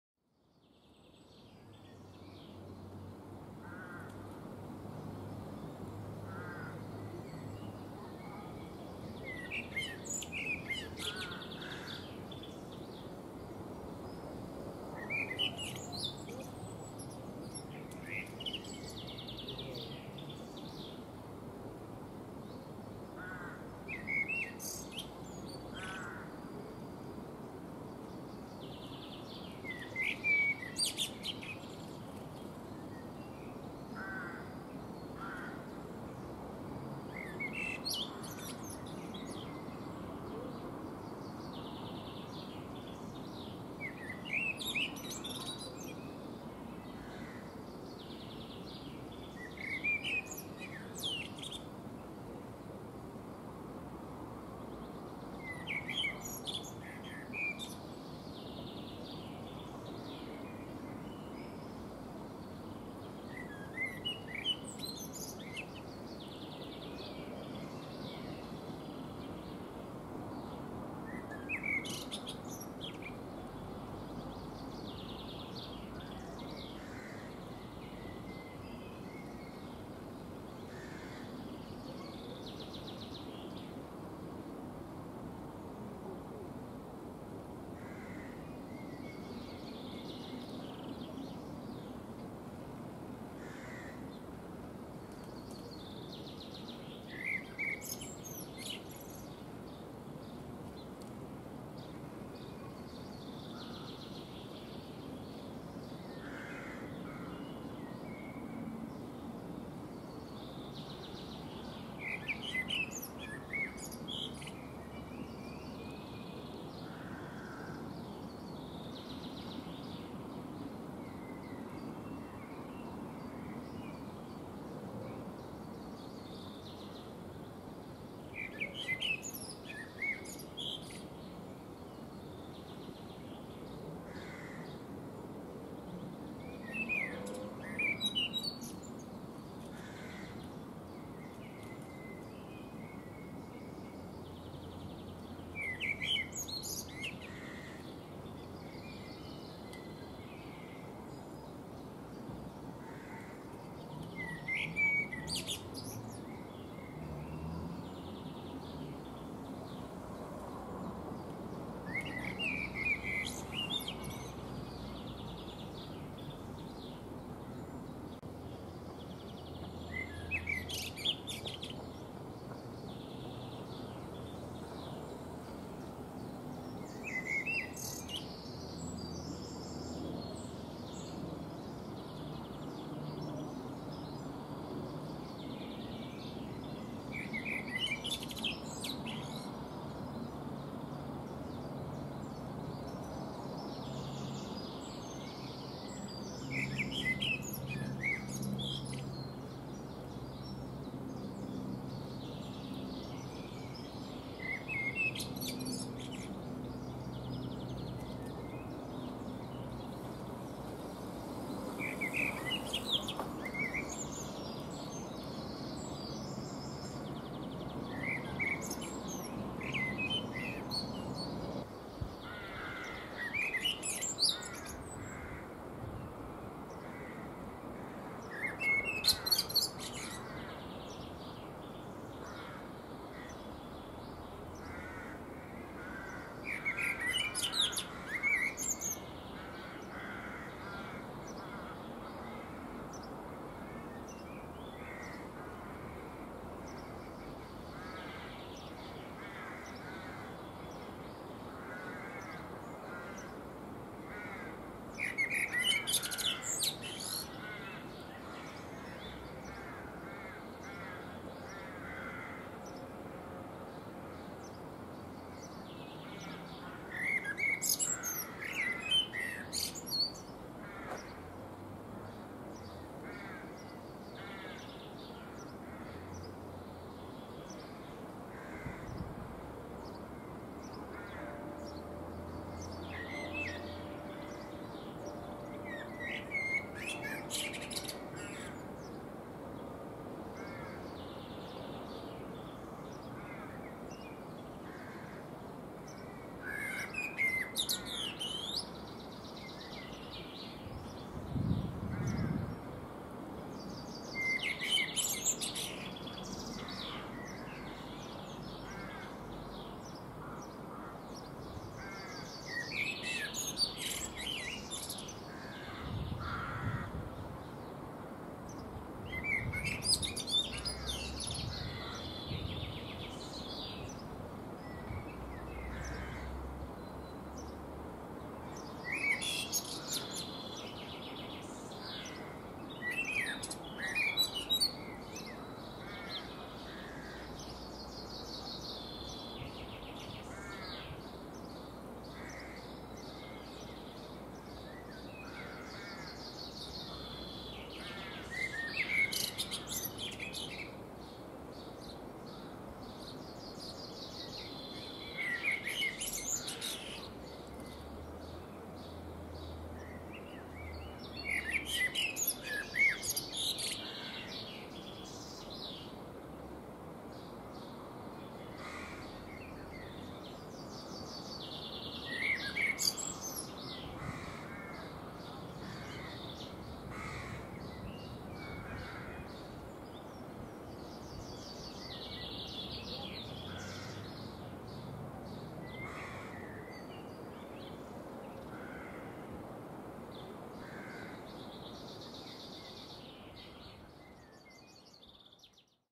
Blackbird Singing And Preening on a Fruit Tree / Singende Amsel
Finally I succeeded to capture a singing blackbird. The recording isn't too much disturbed by some noises / Endlich habe ich es geschafft, eine singende Amsel aufzunehmen. Die Aufnahme ist nicht allzu sehr durch Nebengeräusche gestört 😄